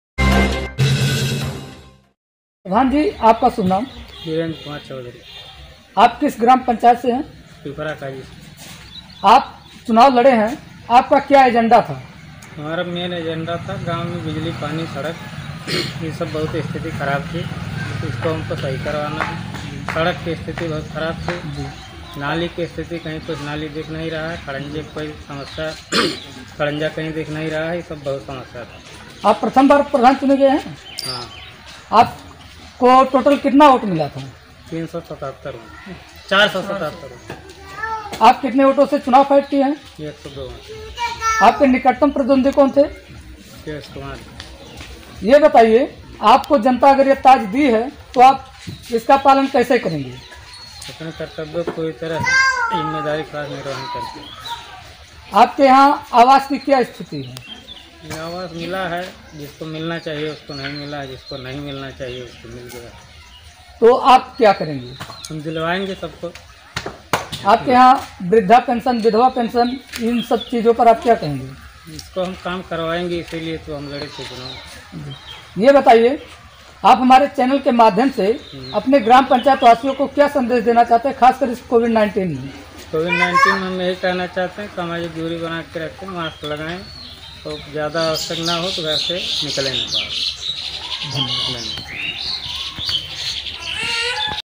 वीरेंद्र कुमार चौधरी ने विकास कार्य को लेकर लड़ा ग्राम पंचायत का चुनाव
बस्ती!

आज हमारी मीडिया टीम ने विकास खंड हरैया के पिपरा काजी ग्राम पंचायत का भ्रमण किया वहां के प्रधान वीरेंद्र कुमार चौधरी से मुलाकात की उन्होंने मुलाकात ने बताया कि मैंने अपने निकटतम प्रतिद्वंदी कृष्ण कुमार को हराकर विजय प्राप्त की है वीरेंद्र कुमार चौधरी ने बताया कि यह मेरी जीत नहीं है मेरे ग्राम पंचायत वासियों की जीत है यहां के हर एक व्यक्तियों की जीत है मैं एक सेवक के रूप में इस ग्राम पंचायत की सेवा करूंगा उन्होंने बताया कि मेरे ग्राम पंचायत की स्थिति काफी दयनीय है, उन्होंने बताया मेरे ग्राम पंचायत में सड़क और नाली की स्थिति काफी खराब है बरसात के दिनों में सड़कों पर चलना काफी कठिन काम है, वीरेंद्र कुमार चौधरी ने बताया कि यदि जनता ने मुझे ग्राम का सेवक चुना है तो मै उसका पालन करूंगा और अपने ग्राम पंचायत का विकास करूंगा क्योंकि मेरा एक ही लक्ष्य है "साफ सुंदर हो गांव अपना" इसी को लेकर अपने गांव का तथा अपने ग्राम पंचायत का विकास  करूंगा जरुरतमंदों को आवास, विधवा पेंशन, वृद्धा पेंशन आदि सरकारी सुविधाओं से लाभांवित करूंगा तथा मैं अपने ग्राम पंचायत का समग्र विकास करूंगा, कोविड 19 वीरेंद्र कुमार चौधरी ने बताया कि सामाजिक दूरी बनाकर रखें मास्क लगाए जरूरत हो तभी घर से बाहर निकले, उन्होंने बताया कि मैं अपने ग्राम पंचायत में सेनेटाइजर का भी छिड़काव करा चुका हूं लोगों से यही कहूंगा साफ-सफाई का विशेष ध्यान दें मैं अपने ग्राम पंचायत वासियों का ह्रदय से हार्दिक, वंदन - अभिनंदन करता हूं!